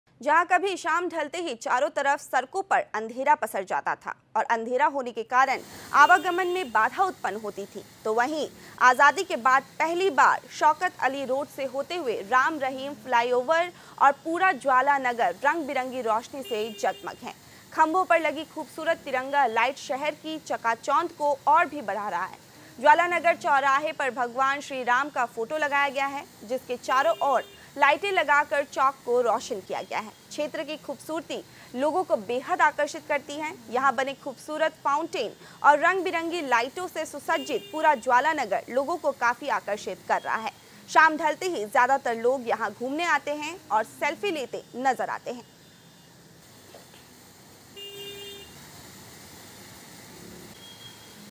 आजादी के बाद पहली बार जगमगाया यूपी का ज्वालानगर…पहले कहलाता था अंधेरी नगरी
आजादी के बाद पहली बार जगमगाया यूपी का ज्वालानगर…पहले कहलाता था अंधेरी नगरी…ज्वालानगर चौराहे पर भगवान श्री राम का लगाया गयाफोटो…चारों ओर लाइटें लगाकर चौक को रोशन किया गया…

pratidintime # HINDINEWS